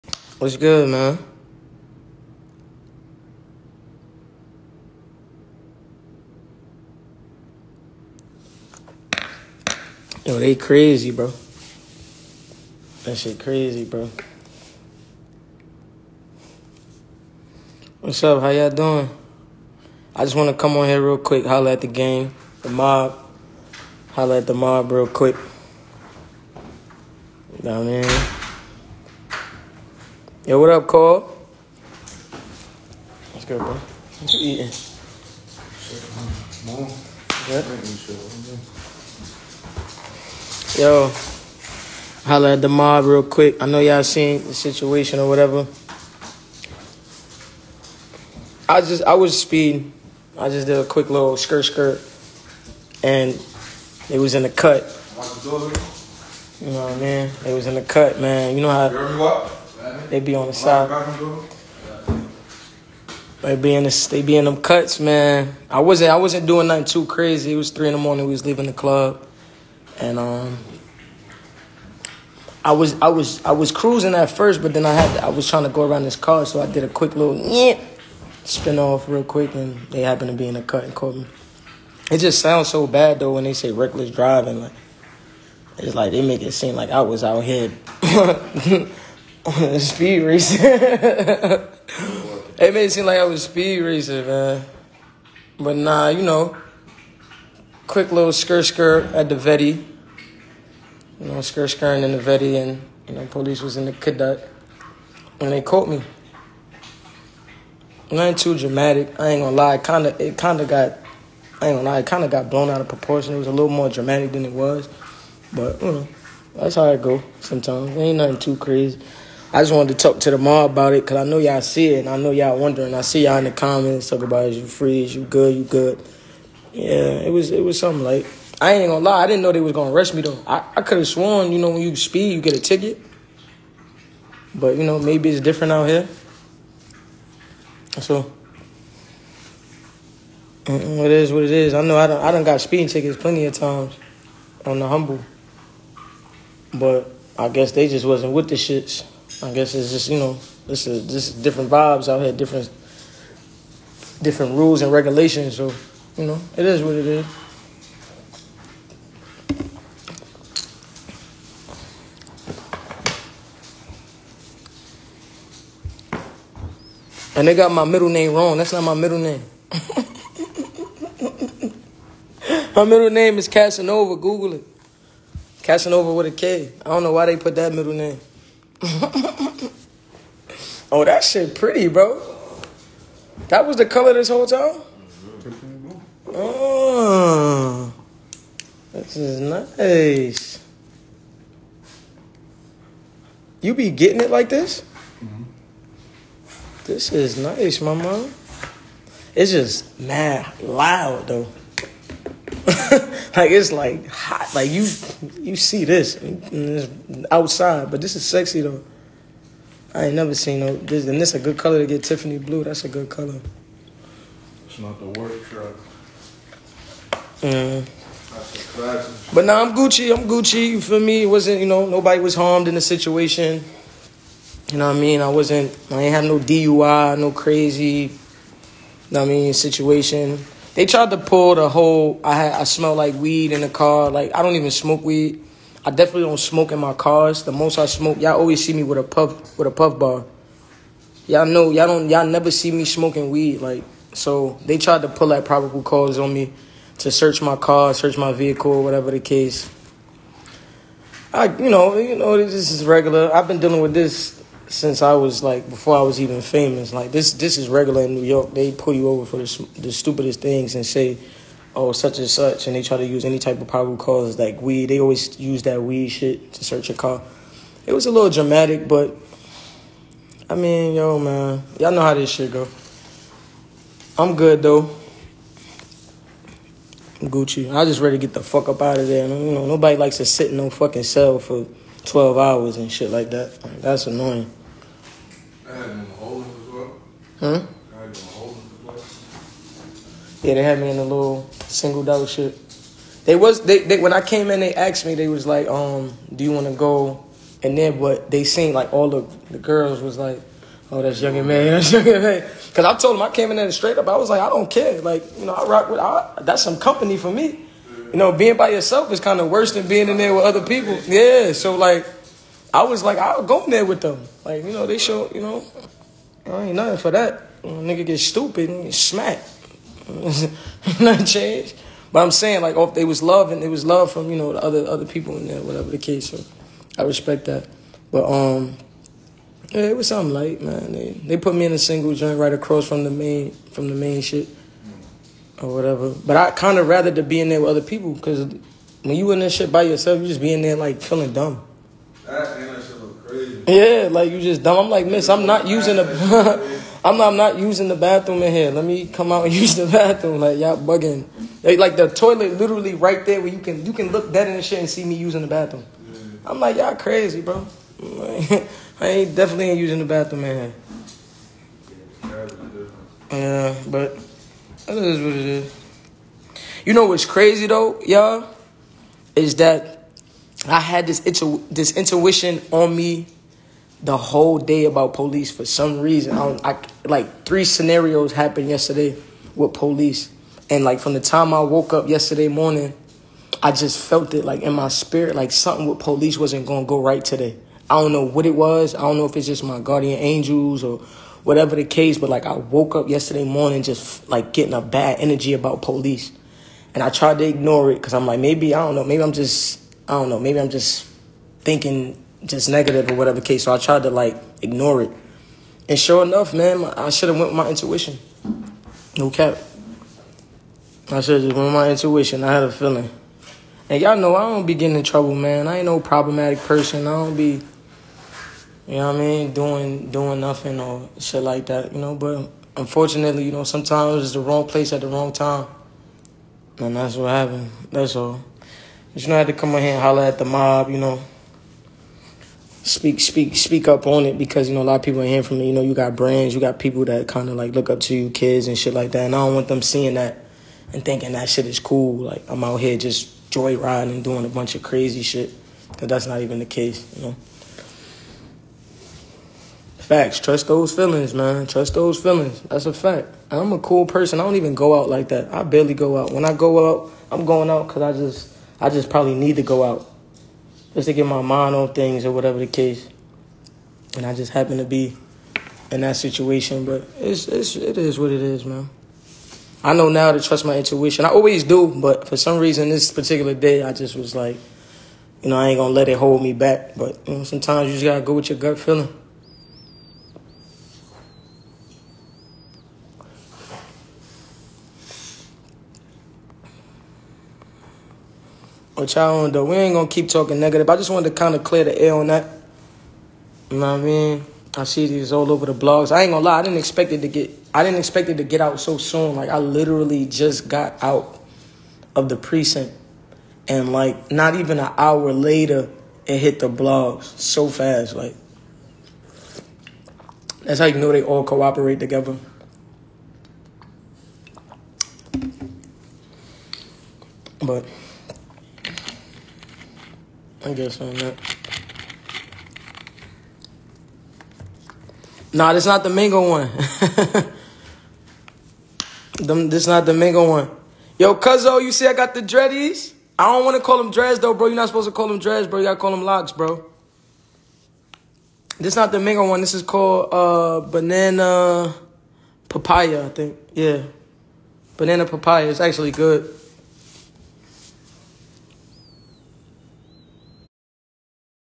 🚨 Young M.A. comes clean about her arrest, shares feelings about dirty cops, and social media blogs
Young M.A. comes clean about her arrest, shares feelings about dirty cops, life in jail, how inmates recognized her while incarcerated, and social media blogs